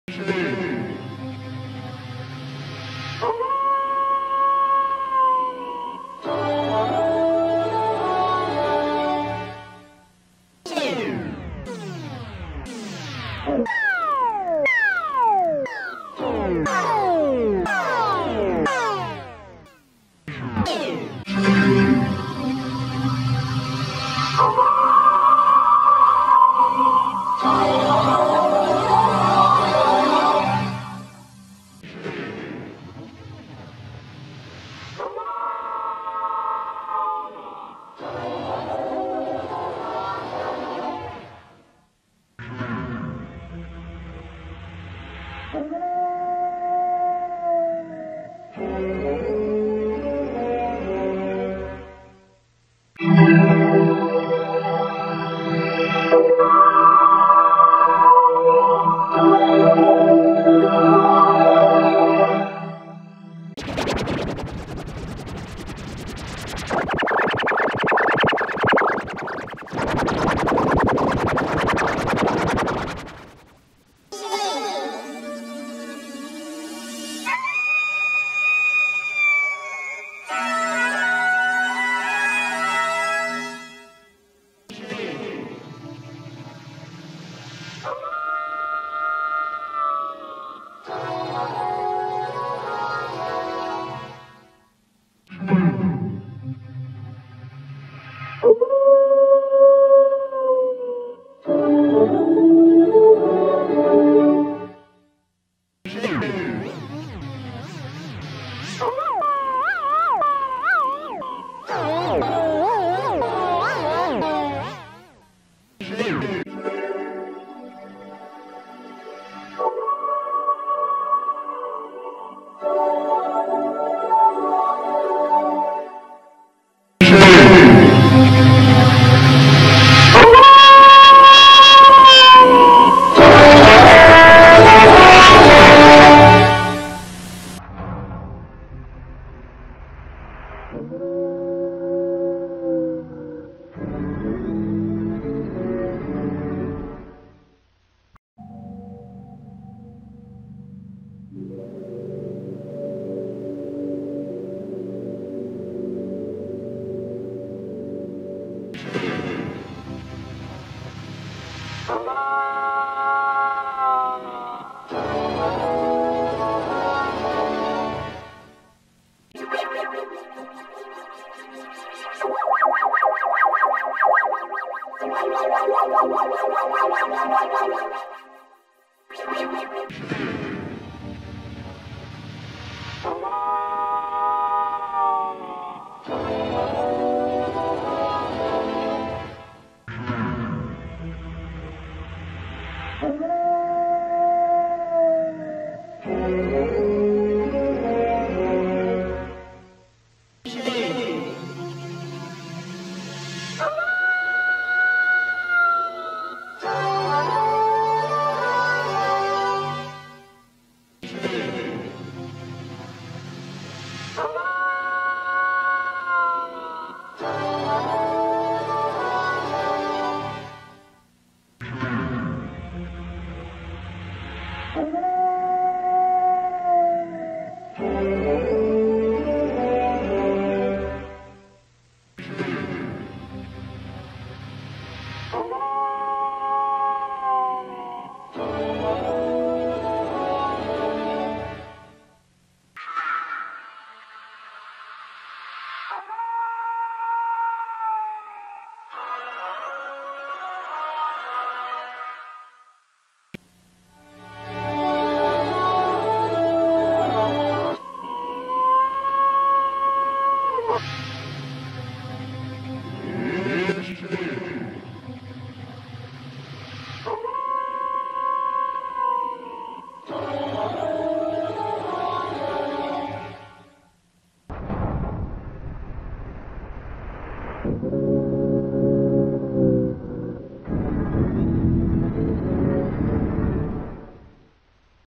Wolf Films Universal TV 1992 effects [Inspired by Touchstone Pictures 2002 effects]
Requested by TorpidAlpaca663

Software used:
VEGAS Pro 17 - Used for video edits
NewBlueFX - Plugin used for video edits
4ormulator - Plugin used for audio edits